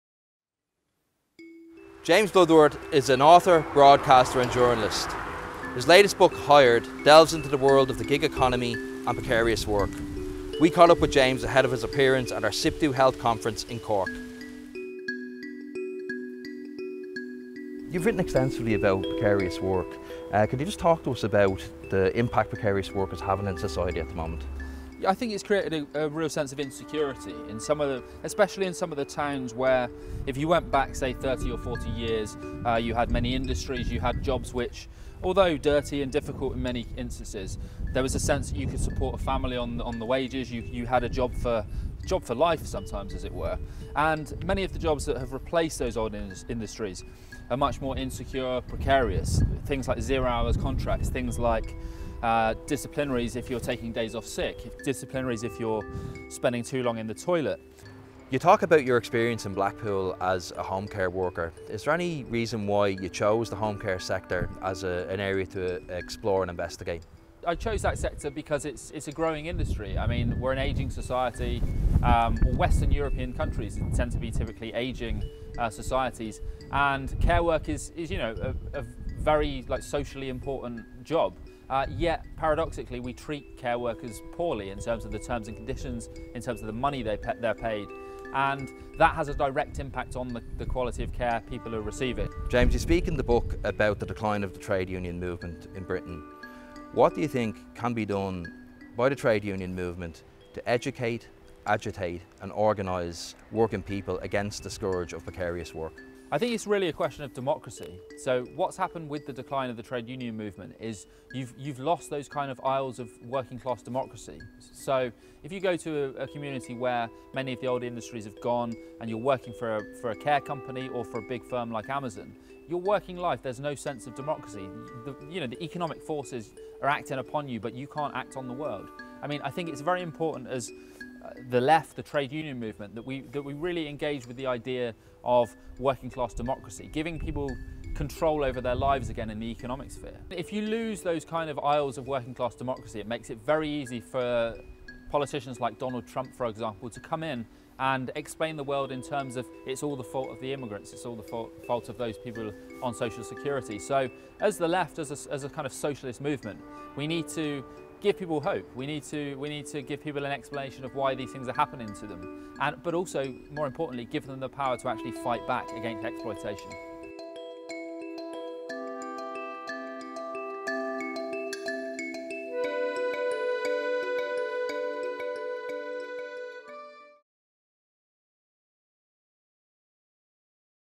SIPTU interview James Bloodworth on the rise of platform and precarious work
To research the oppressive conditions faced by those working in low-paid and minimum-wage jobs, James spent six months working as a care worker in Blackpool, an Uber driver and in the UK packaging warehouse of Amazon.

James Bloodworth was one of the keynote speakers at a SIPTU Health Division Conference.

His book Hired: Six Months Undercover in Low-Wage Britain was published in March 2018.